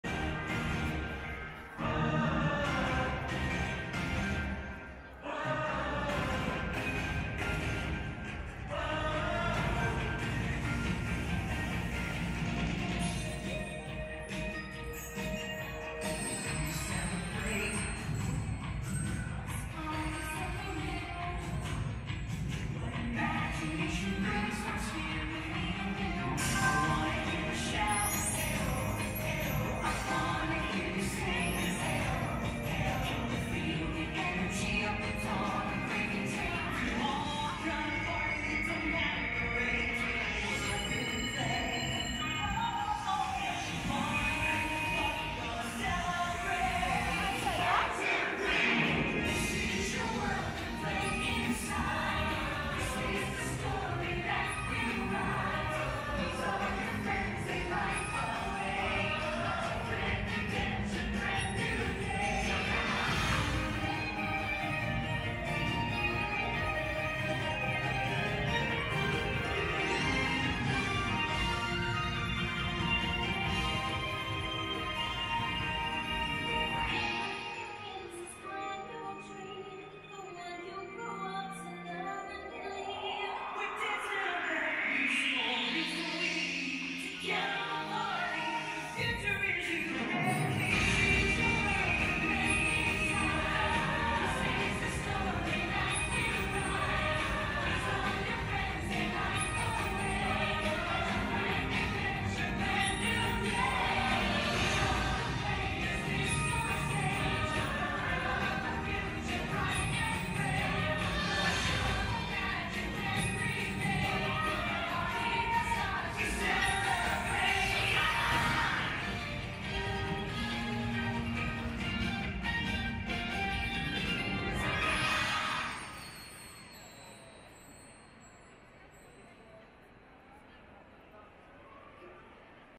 ワールドバザールのプロジェクションマッピング：TDR35th Grand Finale
東京ディズニーリゾート35周年“Happiest Celebration!”グランドフィナーレ時のワールドバザールで夜間に行われるプロジェクションマッピング。「カーズ」、「ファインディング・ニモ」が映るあたり。
DJI OSMO pocketで撮影。